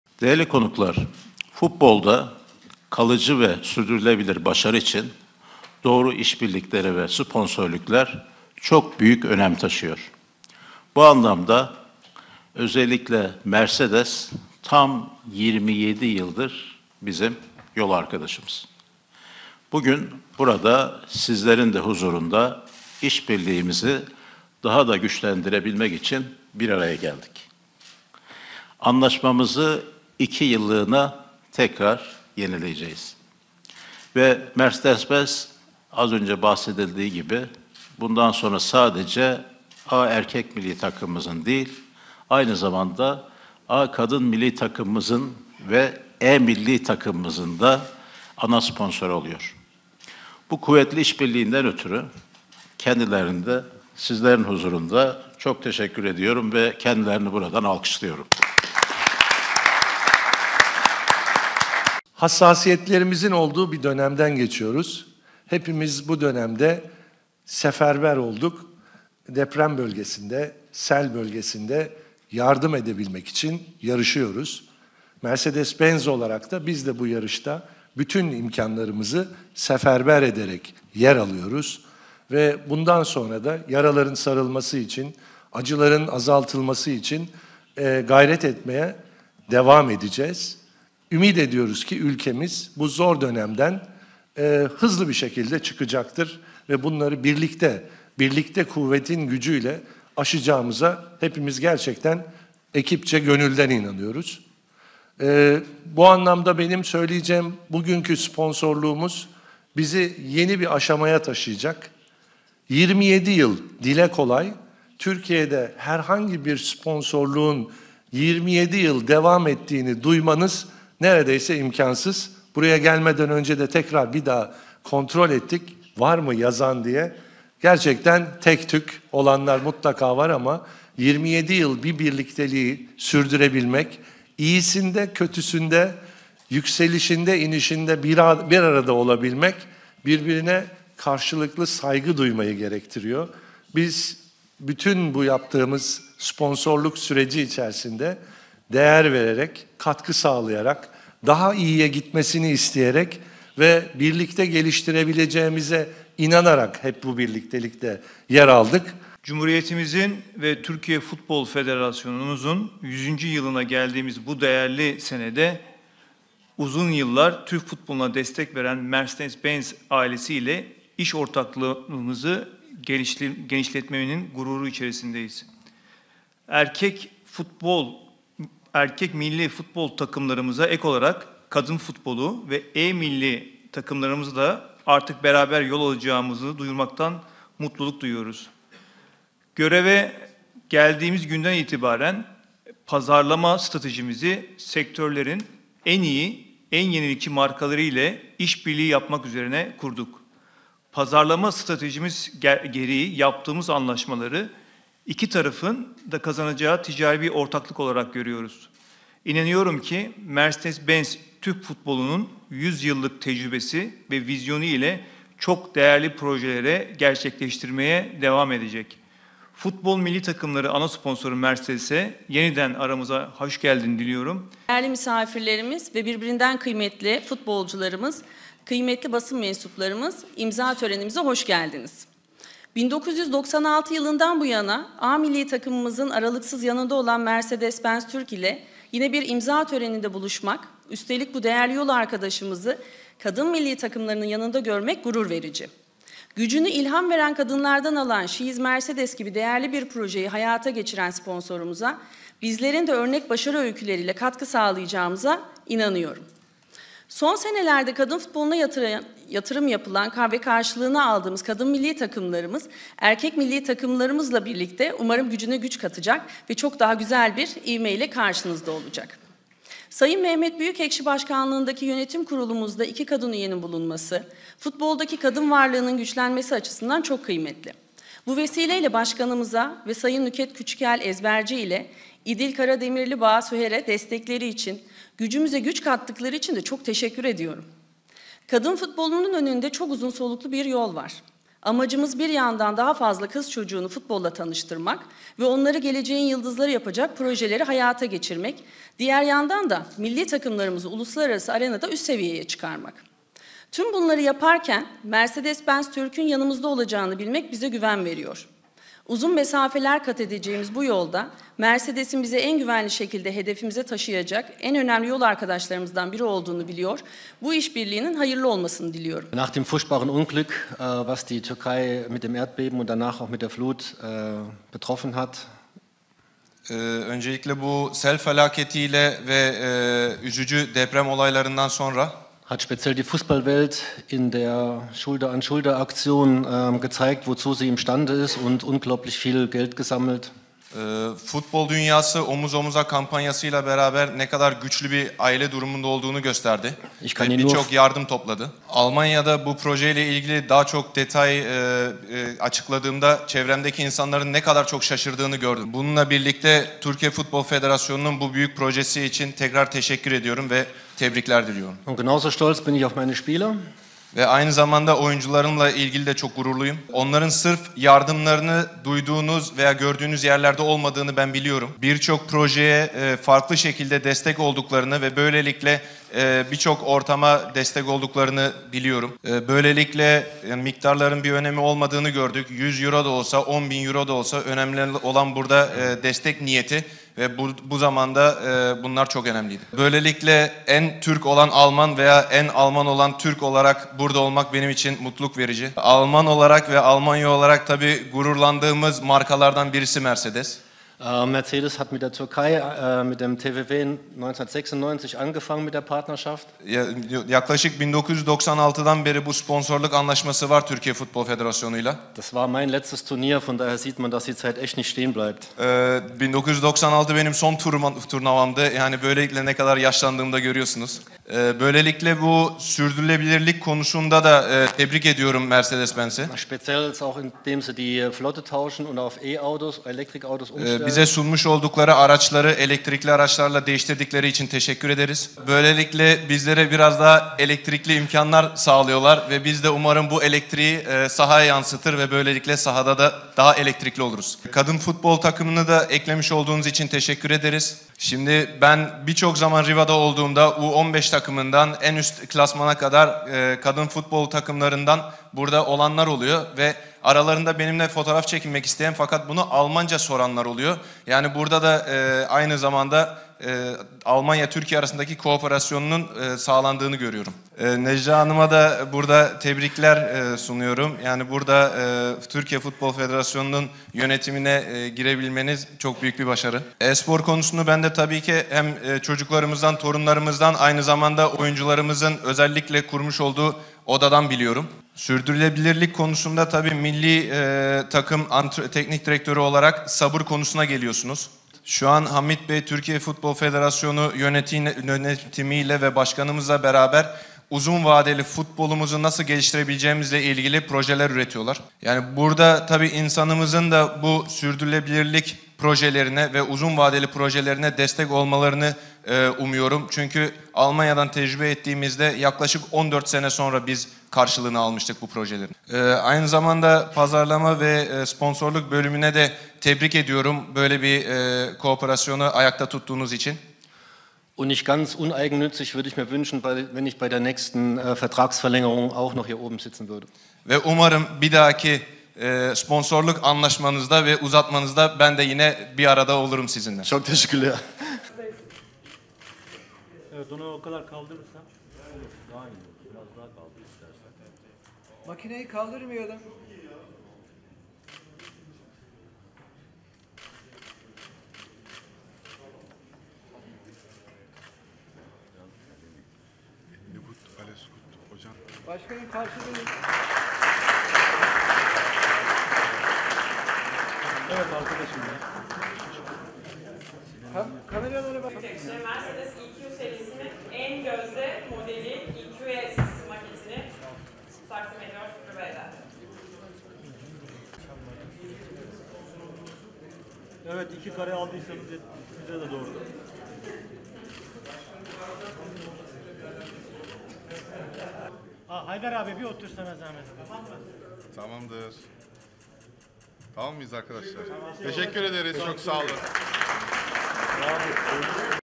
Mercedes-Benz, Milli Takımlar'a resmi ulaşım sponsorluğu ile spora verdiği desteğini sürdürüyor
Mercedes-Benz, Türkiye Futbol Federasyonu Milli Takımlar resmi ulaşım sponsorluğu ile spora verdiği desteği sürdürüyor. Mercedes-Benz, sponsorluk sözleşmesine, Kadın Milli Futbol Takımı ve e-Milli Futbol Takımı’nı da dahil ederek 2 yıl süreyle yeniledi. TFF Başkanı Mehmet Büyükekşi, “Mercedes-Benz’le birlikte yıldızımızın daha da parlayacağına inanıyorum” dedi.